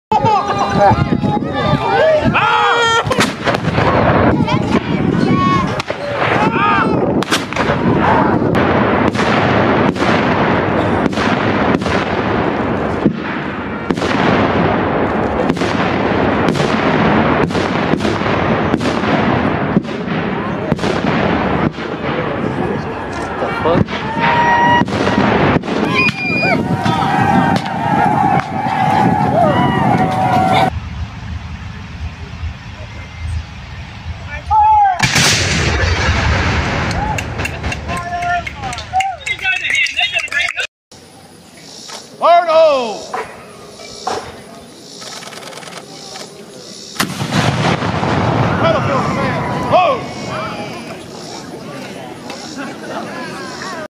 Witness History with this Incredible Cannon Demonstration
Witness history with this incredible cannon demonstration. Cannonballs fly through the air at incredibly high speeds and smash into targets. This is an incredible display of artillery power that will leave you spellbound.

In this video, you'll get a chance to see history in action as the cannonballs fly through the air at incredible speeds. Watch as the balls smash into targets, leaving you spellbound with the incredible power of artillery. This is an incredible demonstration of history and power that will leave you in awe.